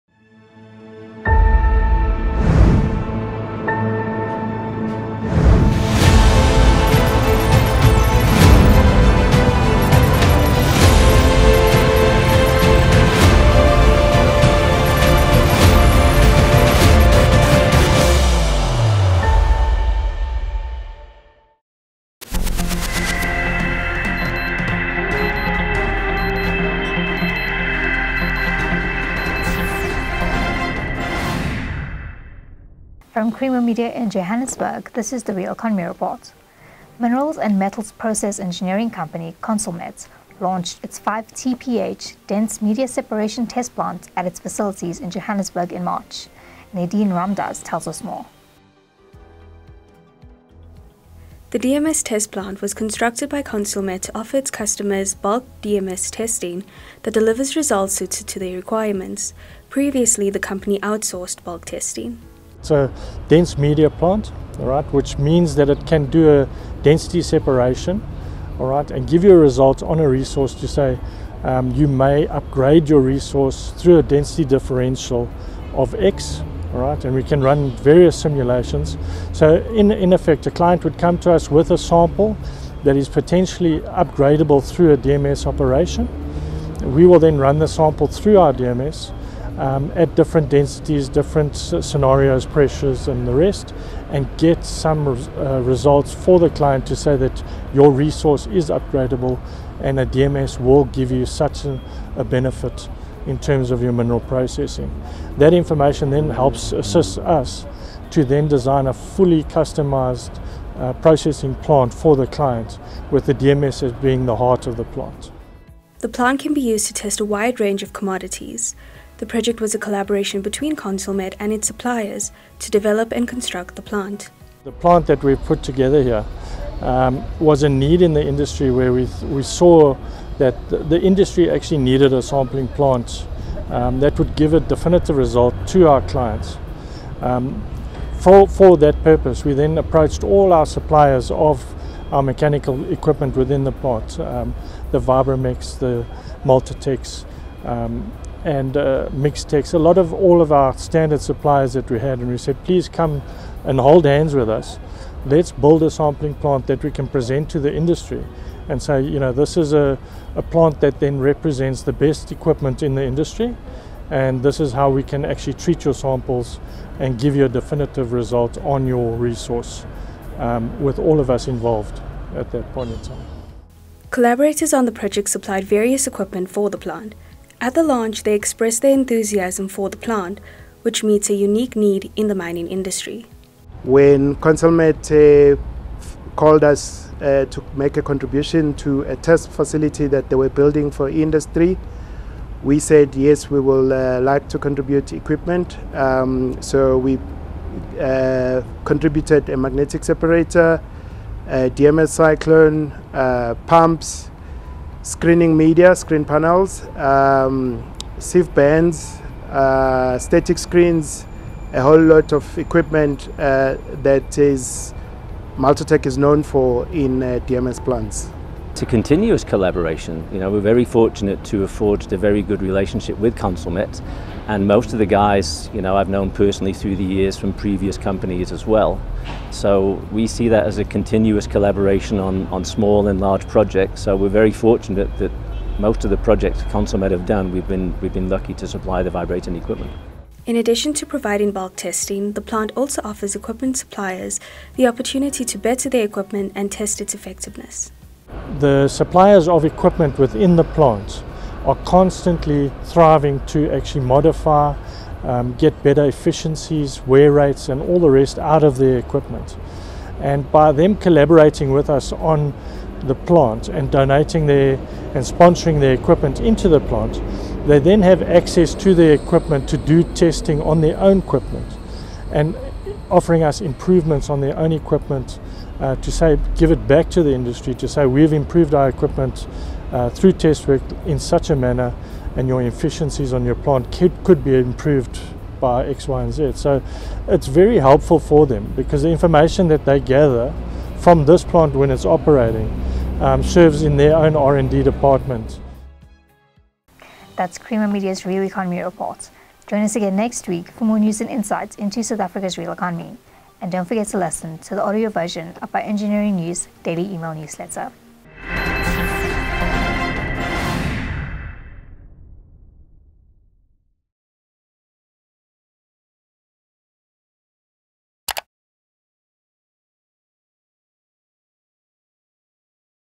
Consulmet launches 5TPH DMS test plant
Minerals and metals process engineering company Consulmet launched its 5TPH Dense Media Separation (DMS) test plant at its facilities in Modderfontein, in Johannesburg, in March.